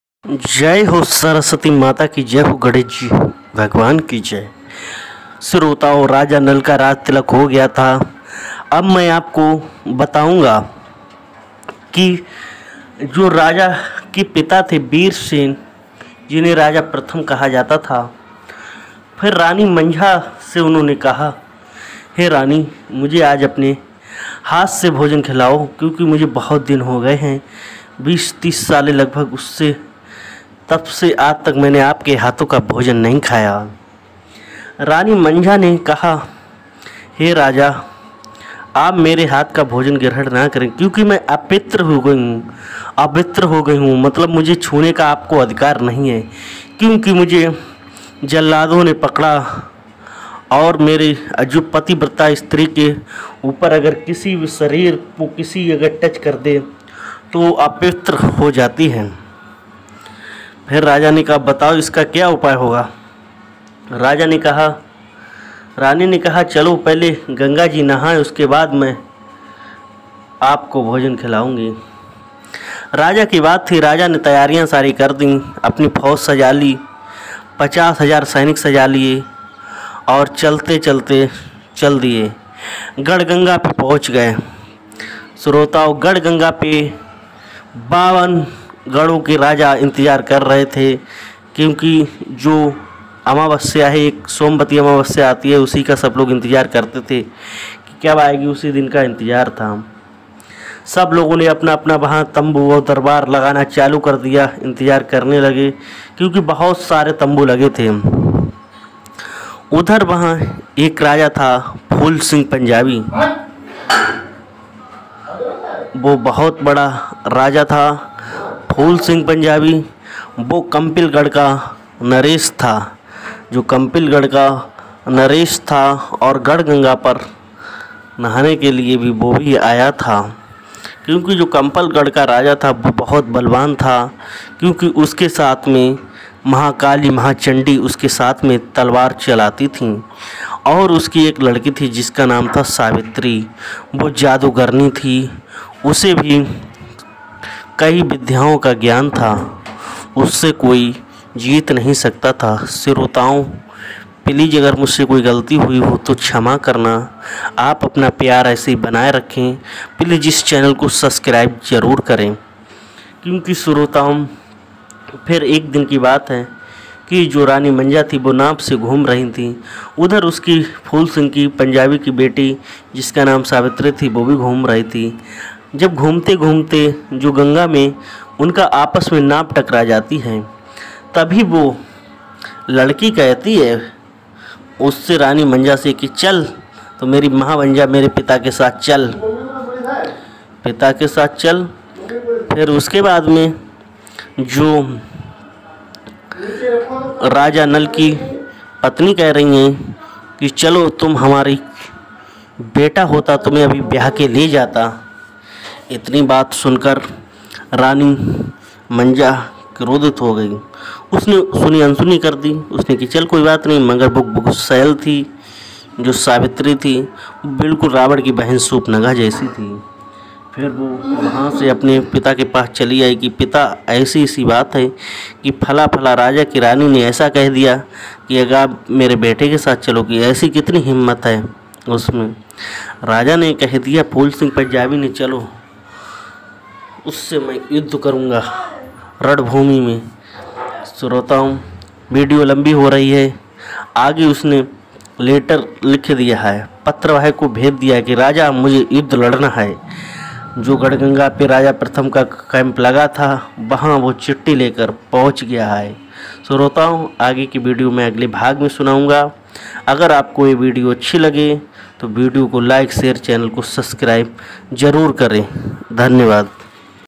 Rani manjani Apne hath se bhojan kyon Nahin karaya Apne Raja Ko bhag 10
राजा नल का राजतिलक होने के बाद में राजा ने अपने रानी मंझा से कहा कि मुझे अपने हाथों से भोजन खिला और रानी मंझा ने कहा मैं आप को भोजन नहीं खिला सकती क्योंकि मैं आप पर रहोगे पहले गंगा जी को नहा कर आओ उसके बाद मैं पवित्र होंगी फिर मैं अपने हाथों से भोजन खिलाऊंगा उधर गंगा नहाने जाती है उधर कंपिल गढ़ के राजा फूलचंद पंजाबी नहीं आती रोड करके राजा को तहस-नहस कर दिया